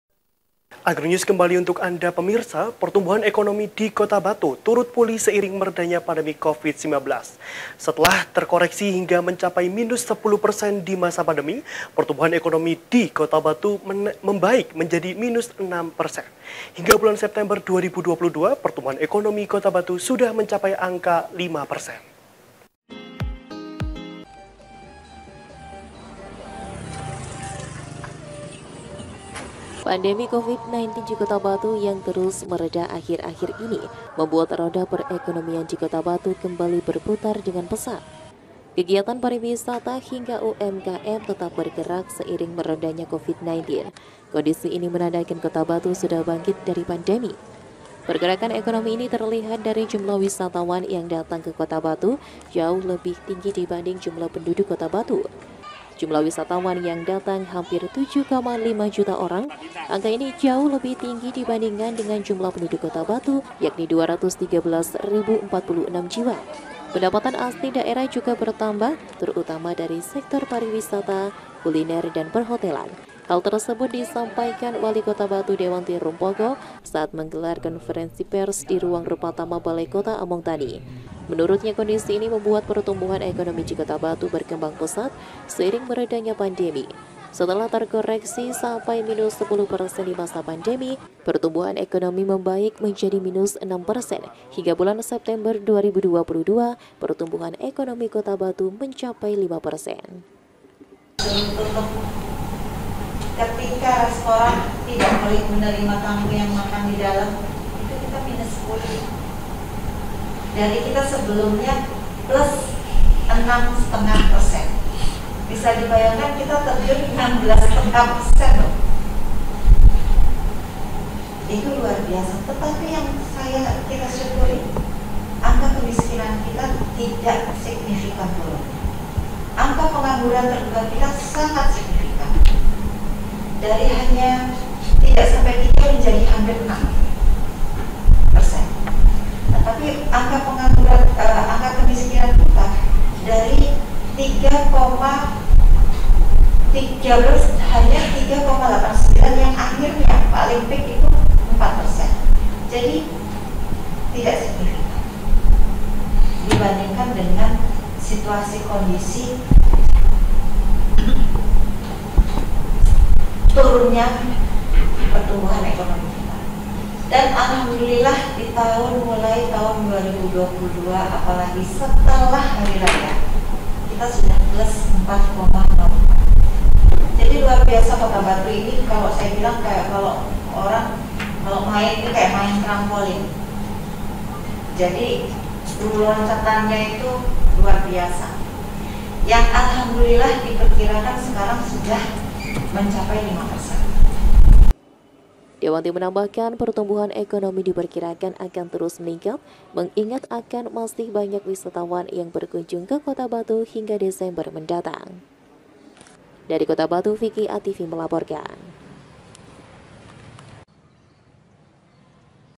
BANGKIT DARI PANDEMI, PERTUMBUHAN EKONOMI DI KOTA BATU MENCAPAI 5 PERSEN | ATV MALANG RAYA
PERTUMBUHAN EKONOMI DI KOTA BATU TURUT PULIH SEIRING MEREDANYA PANDEMI COVID 19. SETELAH TERKOREKSI HINGGA MENCAPAI MINUS 10 PERSEN DI MASA PANDEMI PERTUMBUHAN EKONOMI DI KOTA BATU MEMBAIK MENJADI MINUS 6 PERSEN HINGGA BULAN SEPTEMBER 2022/PERTUMBUHAN EKONOMI KOTA BATU SUDAH MENCAPAI ANGKA 5 PERSEN.